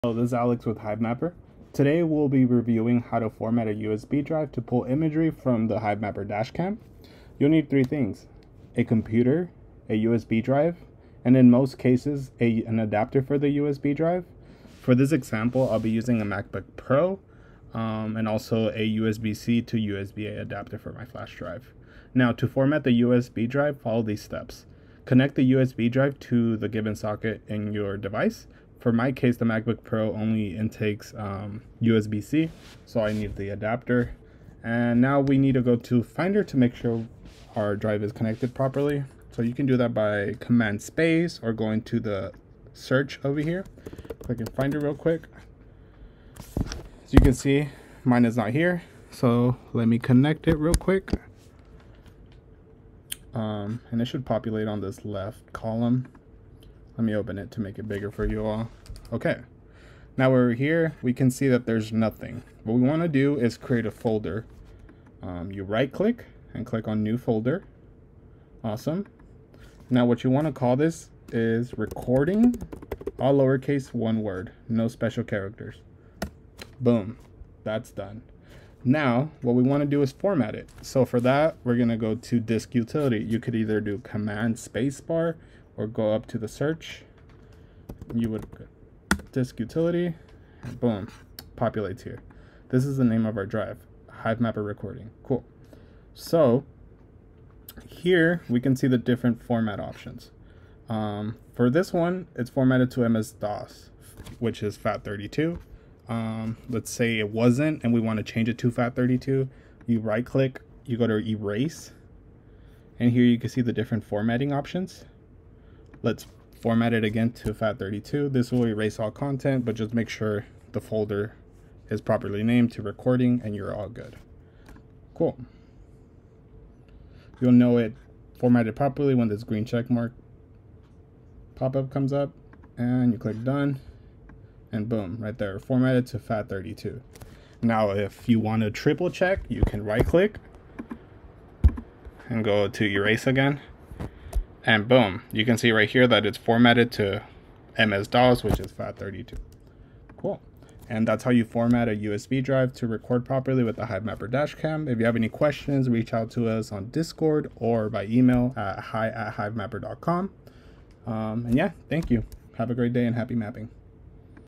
How to Format a USB Drive
Formatting USB Drive in order to pull imagery from Hivemapper Dashcam